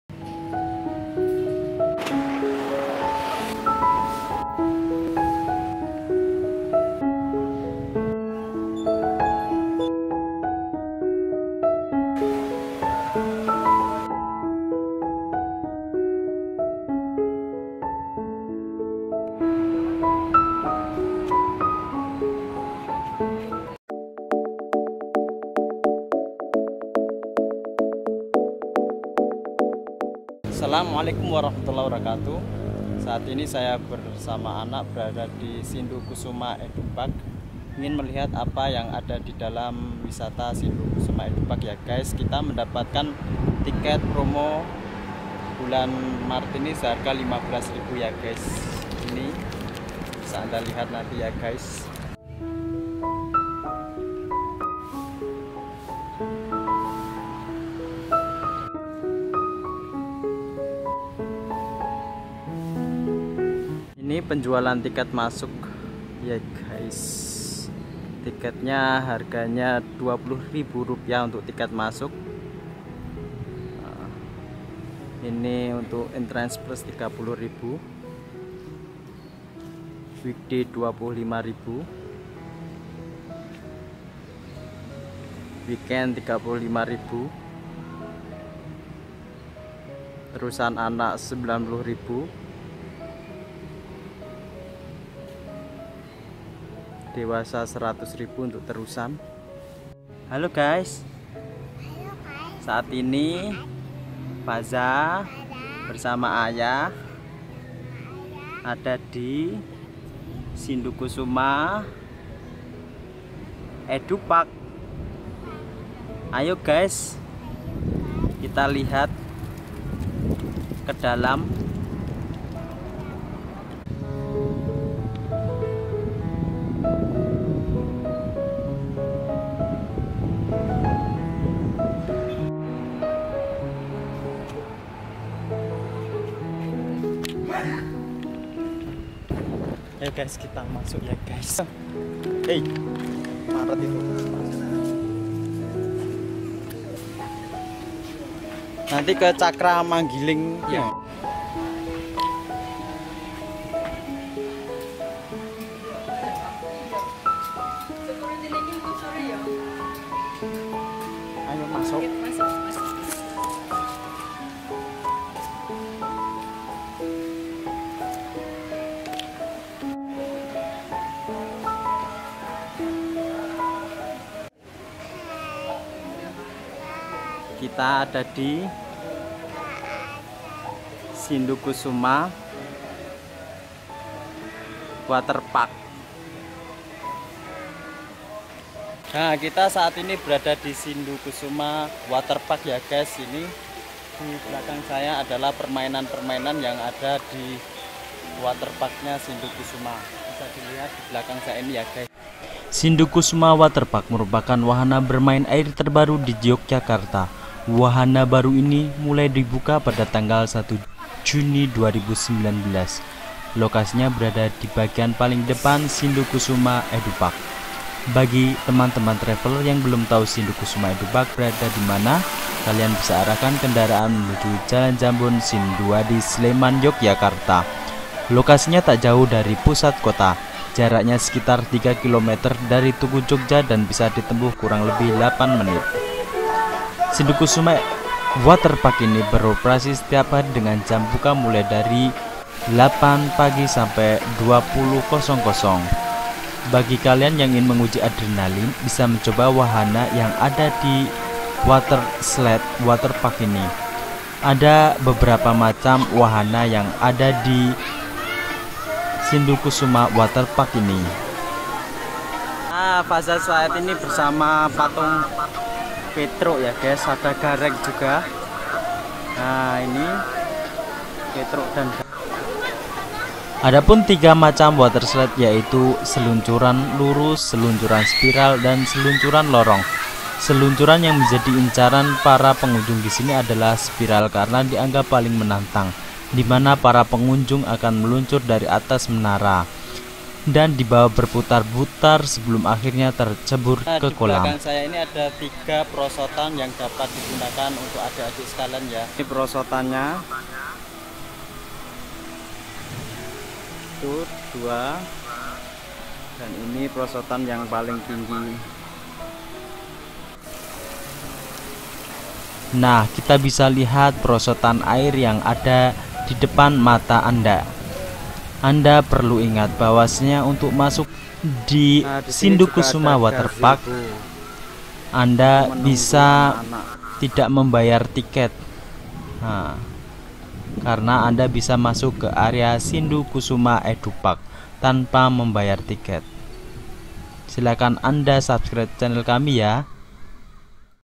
Harga Tiket Masuk SKE JOGJA 2024/2025
Sindu Kusuma Waterpark berada di area yang sama dengan Sindu Kusuma Edupark. Berada di bagian paling depan wahana ini dengan jalur masuk yang berbeda dengan jalur masuk tiket pada umumnya. Namun, jika anda sudah masuk ke sindu kusuma waterpark maka akan secara otomatis diperbolehkan masuk ke sindu kusuma dan dibeberapa wahan yang ada di sindu kusuma

Alamat:  Jln Jambon Jl. Kragilan, Kragilan, Sinduadi, Kec. Mlati, Kabupaten Sleman, Daerah Istimewa Yogyakarta 55284